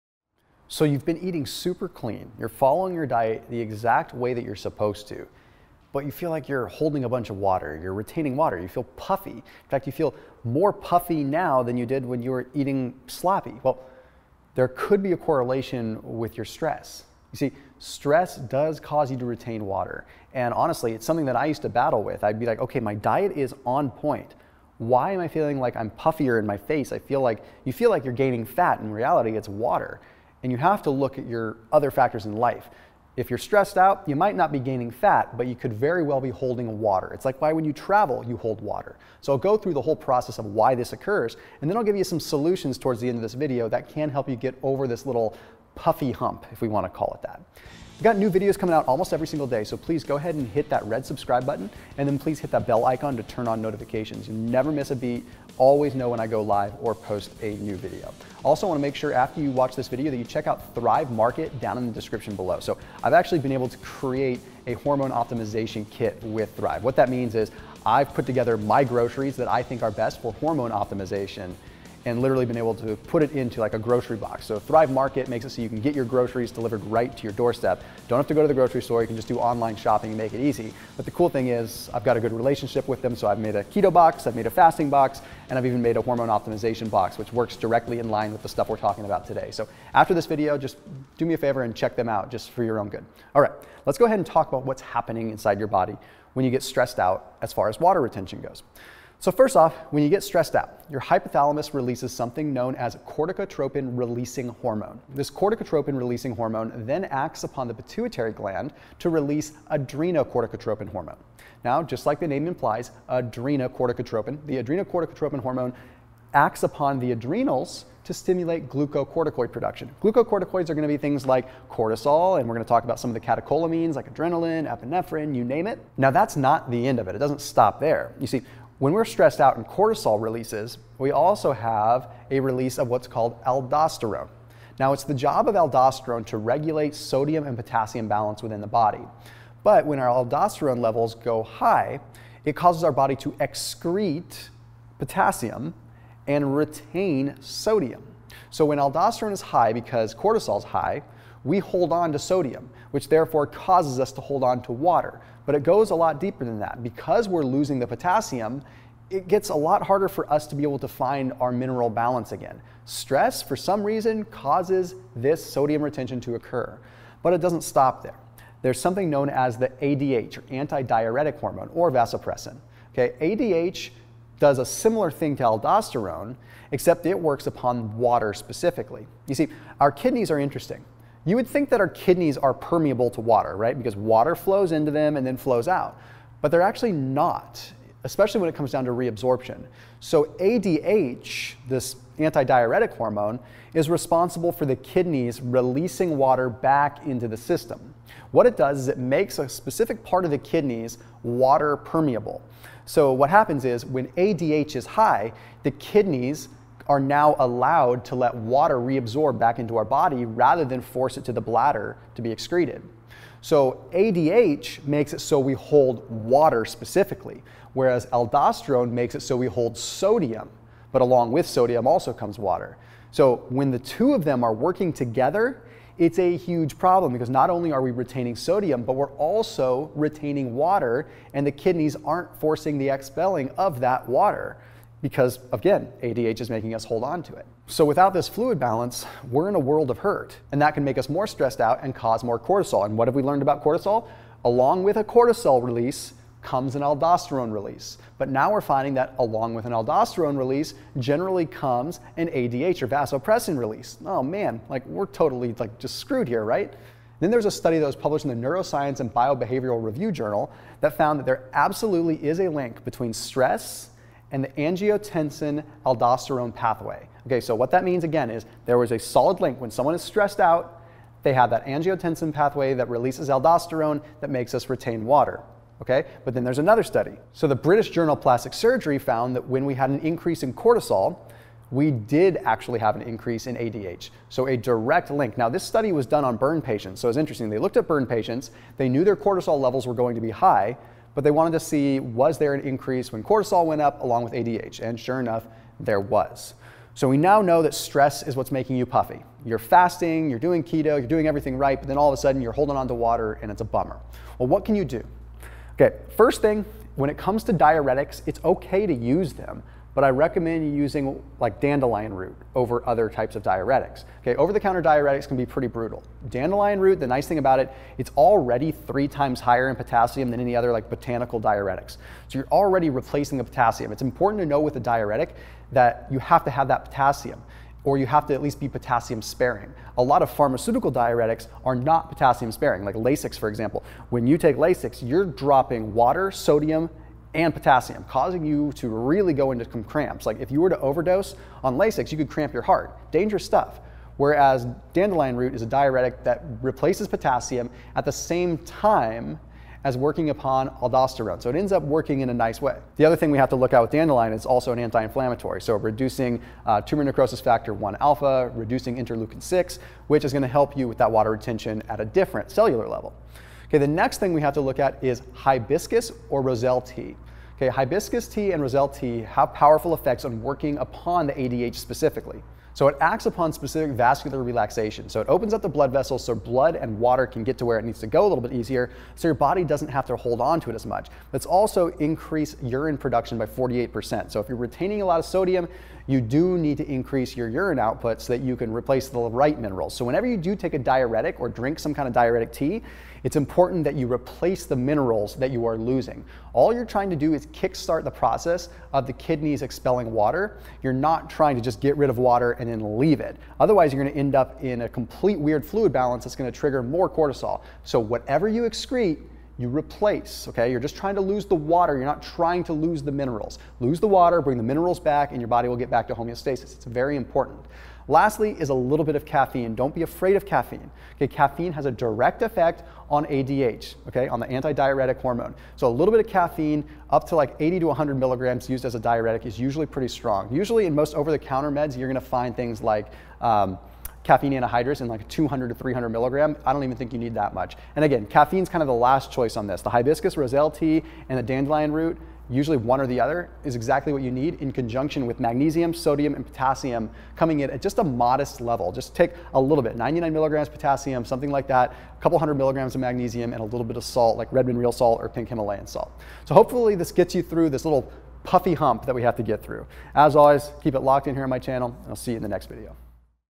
Water Retention- What Makes you Puffy and How to Fix It
Water Retention- What Makes you Puffy and How to Fix It - Thomas DeLauer

Stress & Water Retention

Long-term stress can increase the hormone cortisol, which directly influences fluid retention and water weight

This may occur because stress stimulates cortisol, which, in turn, can cause the release of aldosterone - not only that, but it can also increase ADH

A review published in Neuroscience and Biobehavioral Reviews that looked at the relationship between aldosterone and stress concluded that stress does activate the rennin–angiotensin–aldosterone system

Not only that, but stress reduction studies have resulted in decreases in aldosterone

Additionally, a study published in the British Journal of Plastic Surgery looked at, among other things, the relationship between cortisol and AVP (ADH) in burn patients

Obviously, burn patients had increased levels of cortisol, and researchers found this to be positively correlated with increased levels of ADH

**The exact pathway of how ADH is released is not currently known, but it’s believed that stressful stimuli activate AVP release via parvocellular neurons (part of the hypothalamus) in the paraventricular nucleus, and osmolar stimuli through magnocellular neurons in the supraoptic nucleus** -- these areas are part of the hypothalamus

Hibiscus/Roselle Tea

Roselle tea is an age-old natural remedy for hypertension or high blood pressure. This species of hibiscus contains polyphenols that act as blood pressure-lowering drugs, ACE inhibitors

Roselle also contains quercetin, a flavonoid that contributes to vascular relaxation. It also increased urine production by 48% in a study done on kidney tissue
delphinidin-3-O-sambubioside, 11.5 mg/g cyanidin-3-O-sambubioside, 11.5 mg/g quercetin which have been found to actually control aldosterone activity

A study from the Journal of Ethnopharmacology found that hibiscus tea increased urine production by 48% in a study done on kidney tissue

Coffee/Tea

Technically speaking, caffeine in coffee is a natural diuretic that binds to adenosine receptors - this effect prevents the kidneys from taking up sodium and enhances water and sodium elimination

Renal renal reabsorption requires functional adenosine A1 receptors, which caffeine locks onto - via this effect, caffeine can decrease salt and water reabsorption in distal nephron segments, thereby altering ADH secretion

*Adenosine receptors are expressed along the entire nephron*

Additional Resources